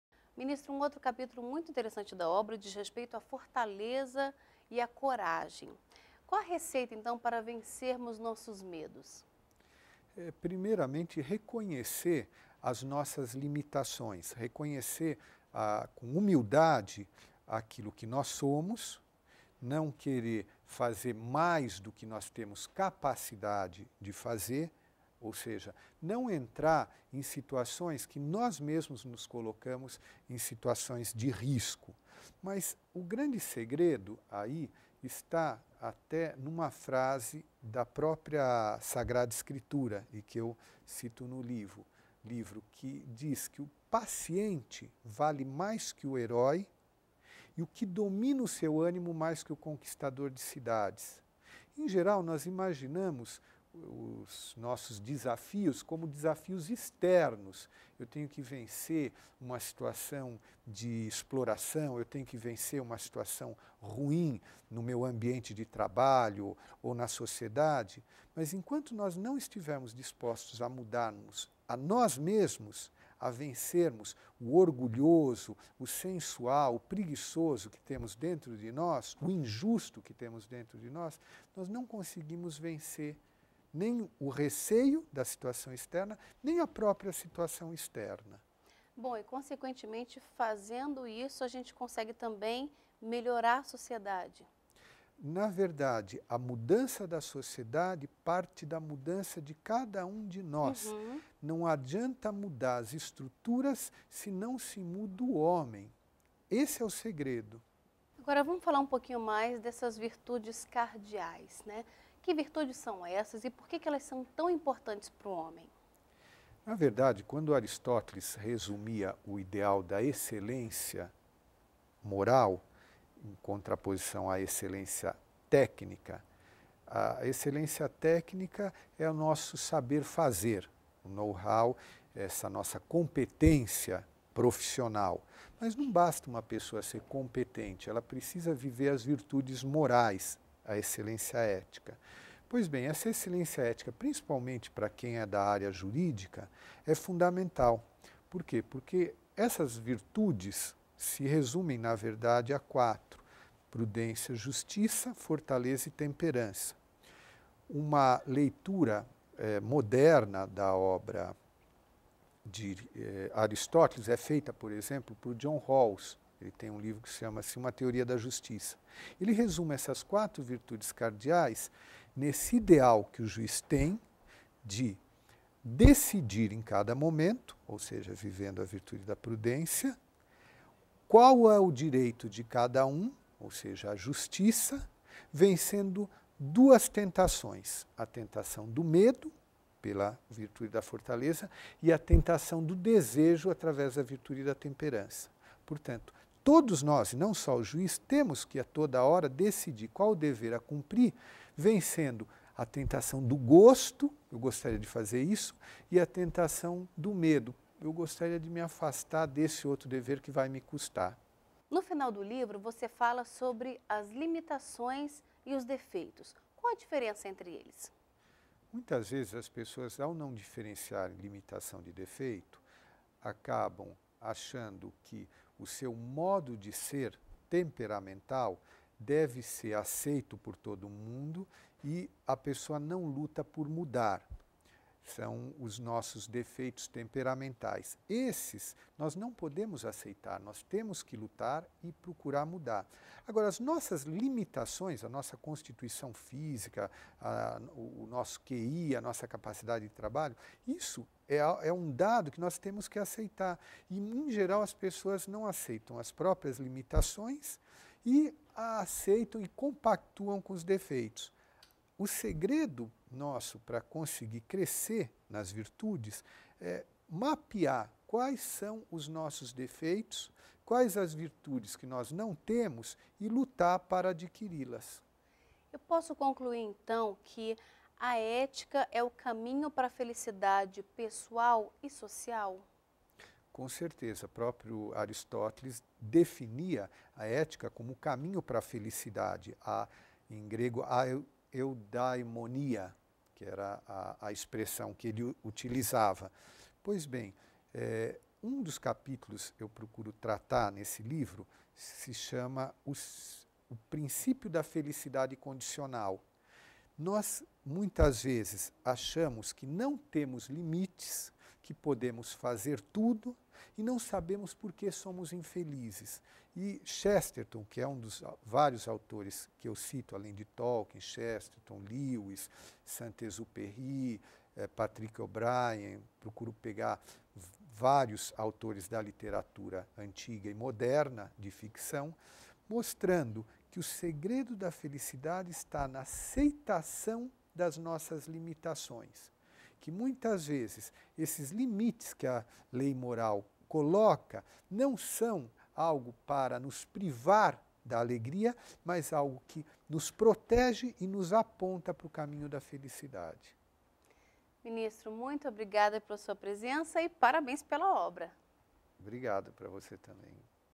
Encontro com o autor - Ives Gandra Martins Filho (2/2)
No quadro Encontro com Autor você vai ver uma entrevista com o Ives Gandra Martins Filho, ministro do Tribunal Superior do Trabalho. Ele vem ao programa falar sobre seu livro: "Ética e Ficção -- de Aristóteles a Tolkien".

Durante a entrevista, o ministro fala da importância da ética no Direito: "Se nós não tivermos uma visão legalista da ética dos deveres, mais uma visão otimista - que é da ética clássica - nós vamos encarar o Código de Ética como um ideal a ser atingido", explica.